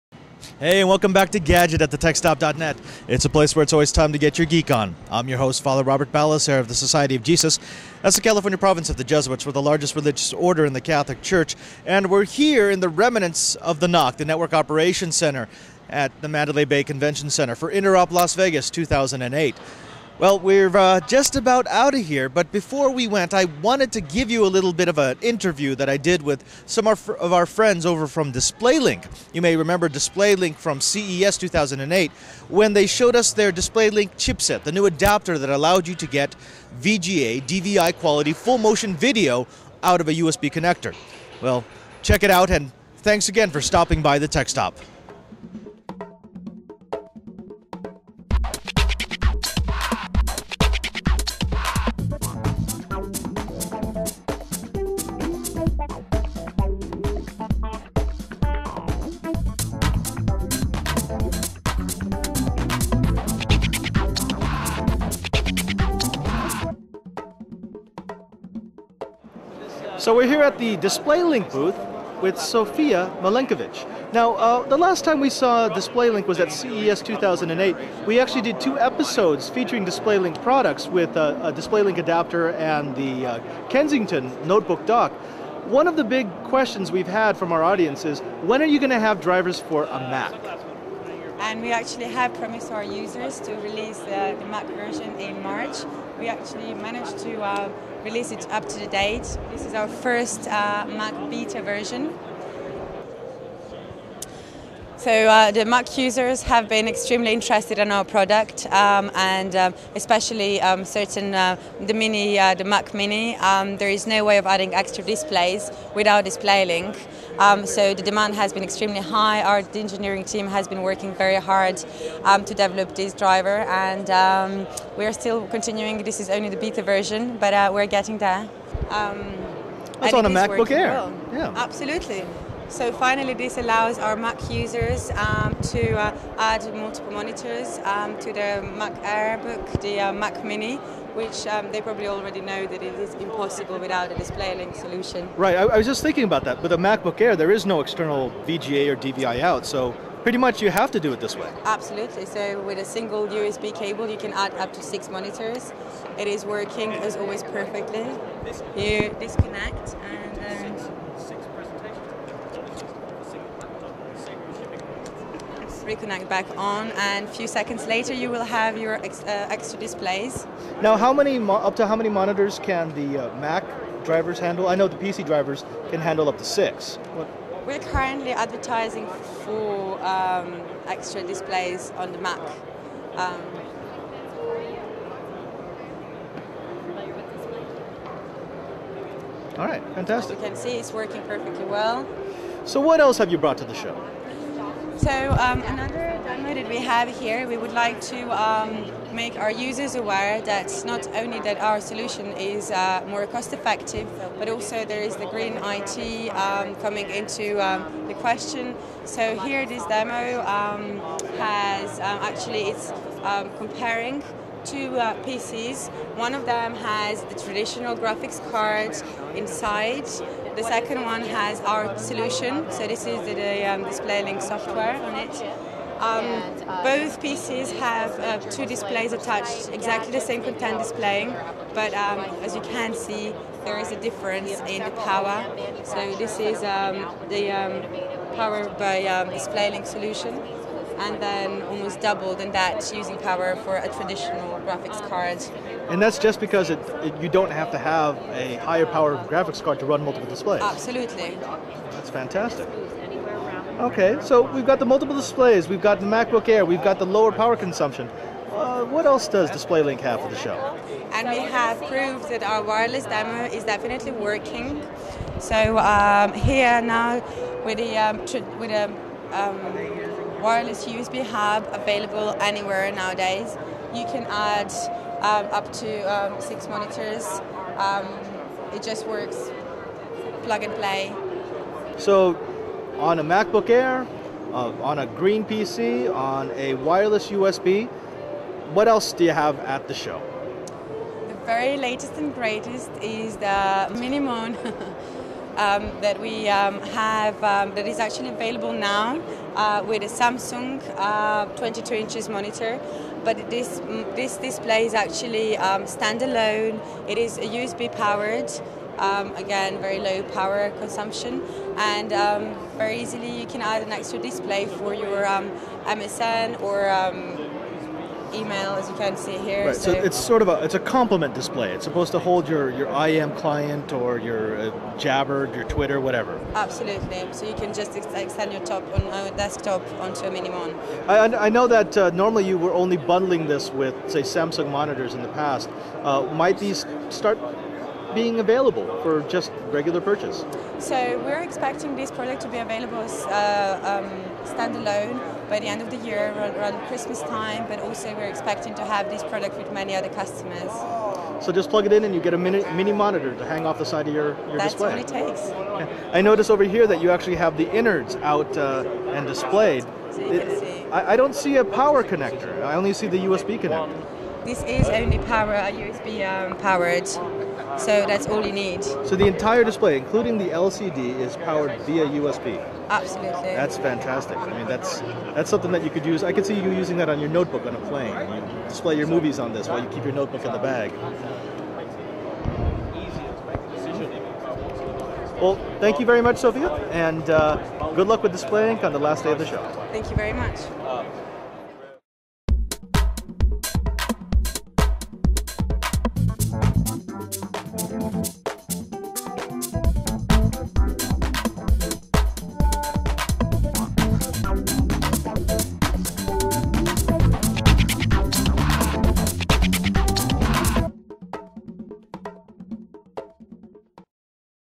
JESUITS - 'gadget' @ TTS - Ep69- DisplayLink @ Interop 2008
The last time we saw DisplayLink was at ShowStoppers @ CES 2008. They've come up with a winner in a chipset that can push high-quality, full-motion video over a standard USB 2.0 interface.

We caught up with them again at Interop Las Vegas 2008 and they had a few more announcements for multi-monitor fans.

You can find more 'gadget' episodes, in high-resolution at